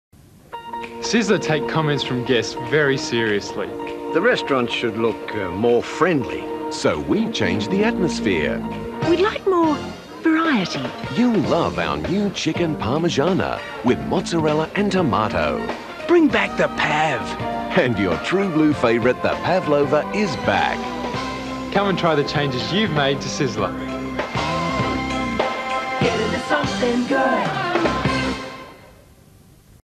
Sizzler "Something Good" - Keiren Perkins (Australian Swimmer) #2
Kieren John Perkins, OAM (born 14 August 1973) is a former Australian freestyle swimmer. He specialised in the 1500-metre freestyle and won successive Olympic gold medals in this event in the 1990s. He won his first at the 1992 Olympics which he won in world record time and then at the 1996 Olympics when he defended his title. In total, he won four Olympic medals.